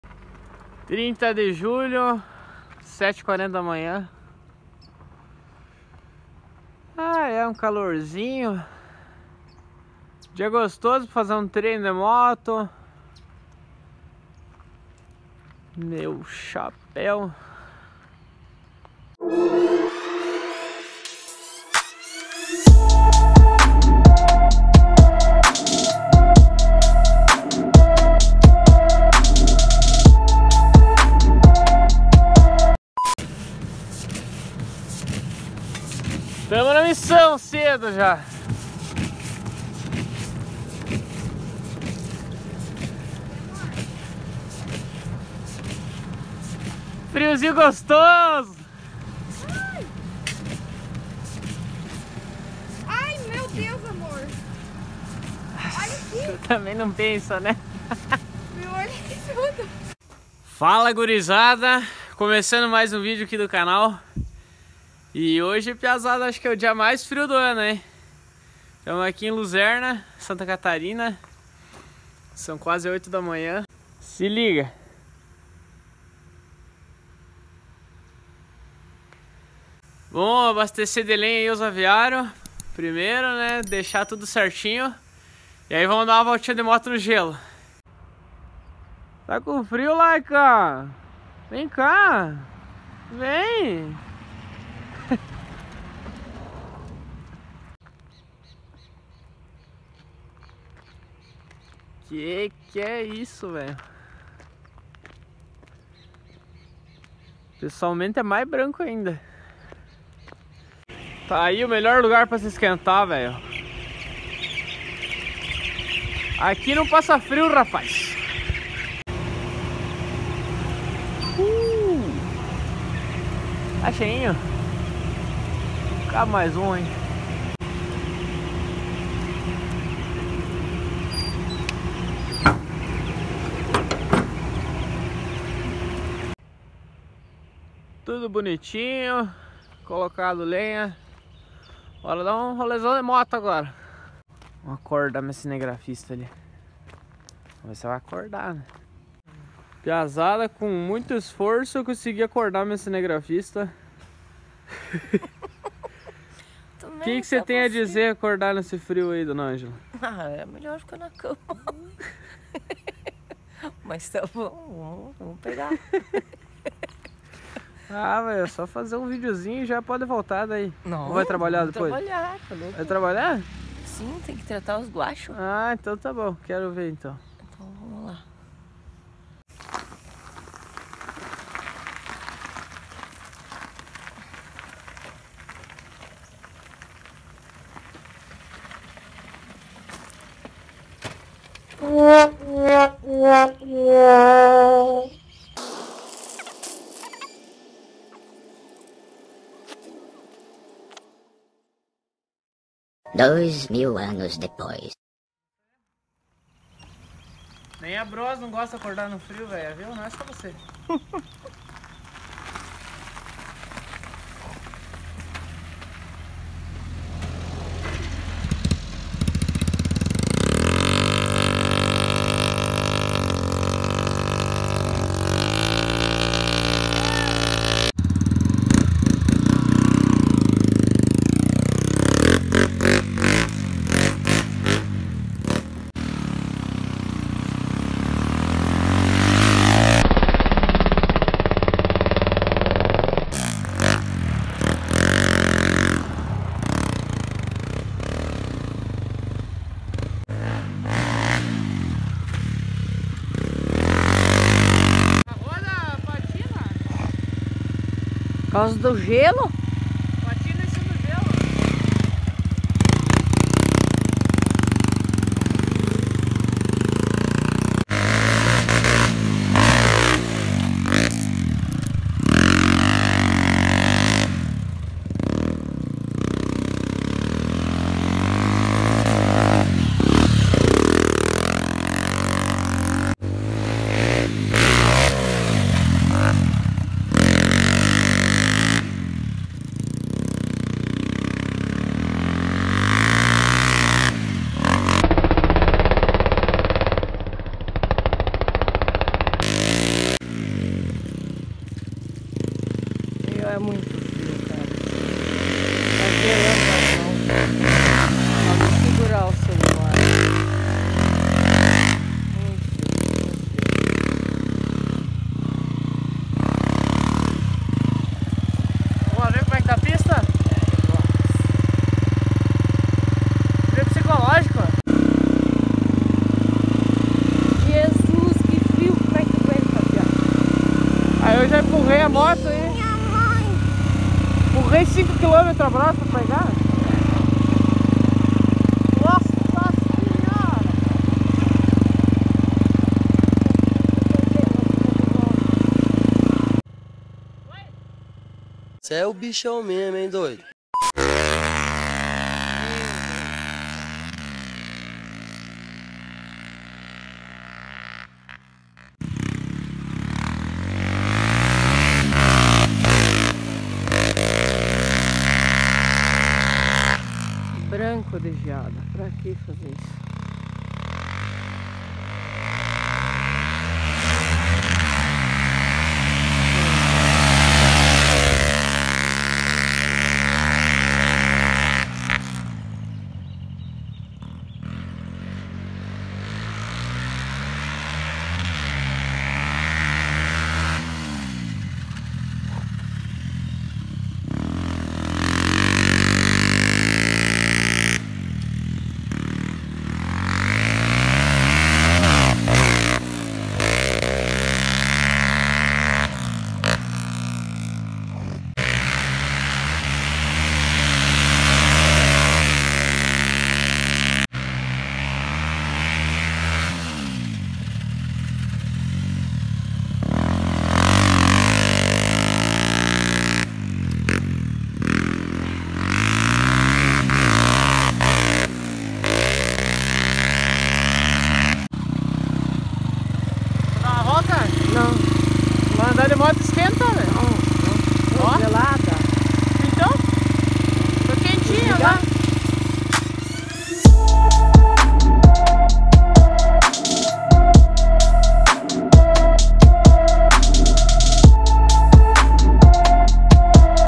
Andando de moto no DIA MAIS FRIO de Santa Catarina!
Hoje foi dia de dar um rolê com a Bros 125 no gelo! O dia mais frio do ano em Luzerna - Santa Catarina.

SUSPENSÃO É BENDER SUSPENSÕES!

OFICINA DE MOTOS É JD MOTOS!

Eduardo Heinfarth #153​​​ 🏍️ Cg 125 1984 | Ktm/Sxf 450 | Bros 125

Não deixa de se inscrever se curtiu o vídeo! ✅